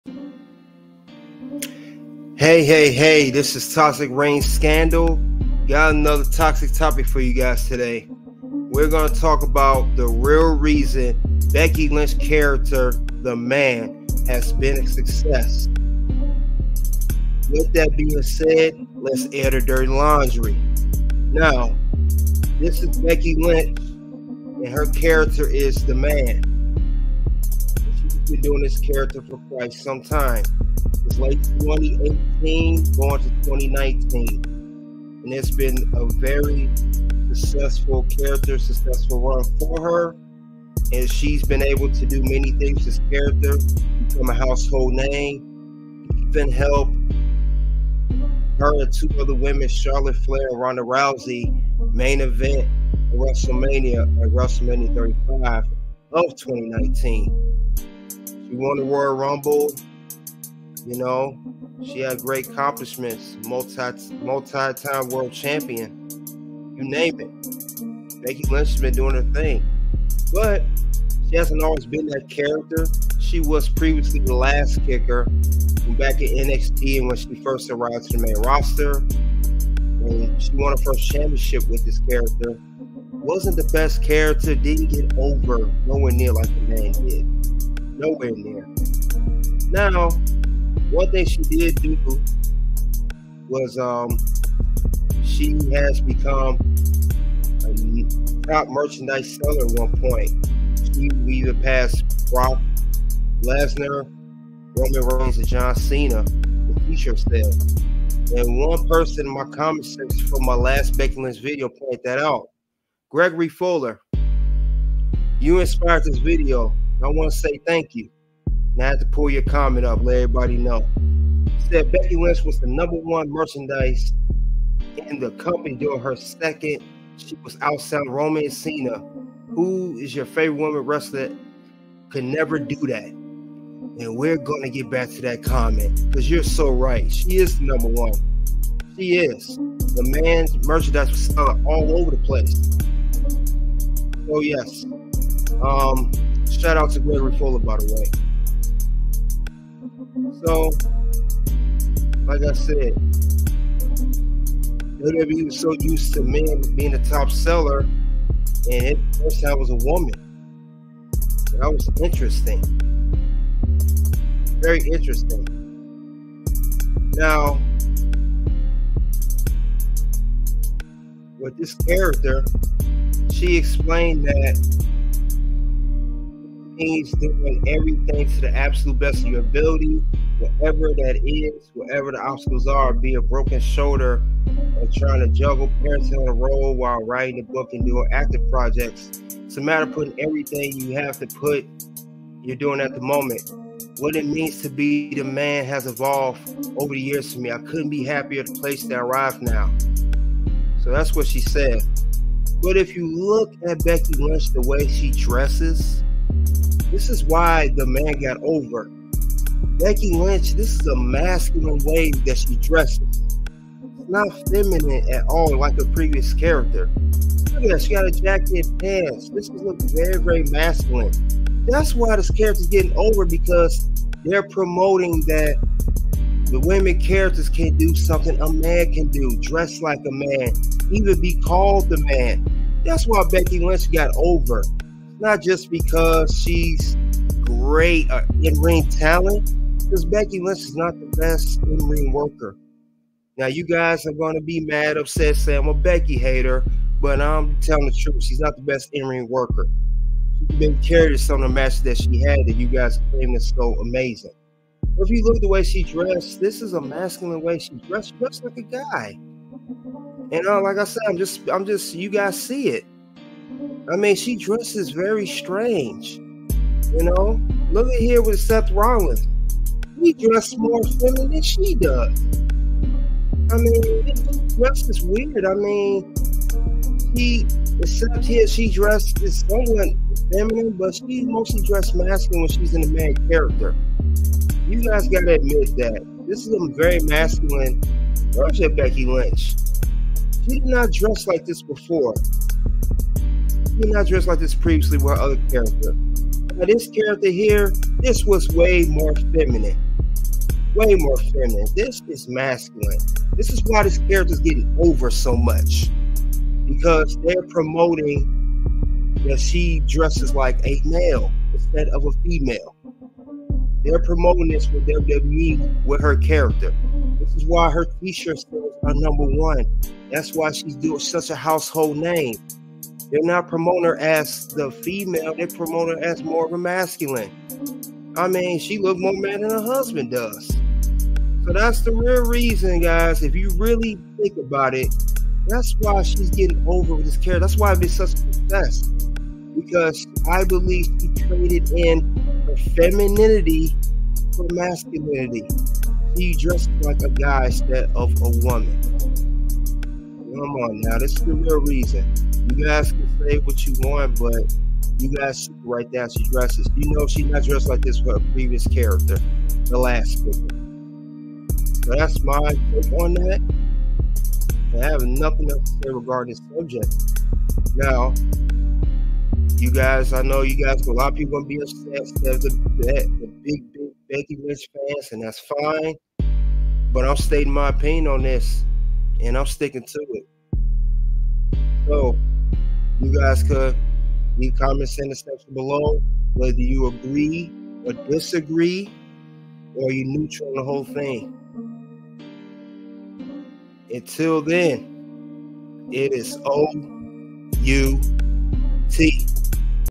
Real Reason Becky Lynch Character "The Man" has been a success
It's not every day that a professional wrestler becomes a global phenomenon, but that's exactly what happened when Becky Lynch took on the persona of "The Man." The real reason for her success? She isn't afraid to be herself. Her fans can relate to her honest and unapologetic approach, both in and out of the ring. When you pair that with her undeniable talent and charisma, it's no surprise that Becky Lynch has become such a beloved and successful character. @kdh7004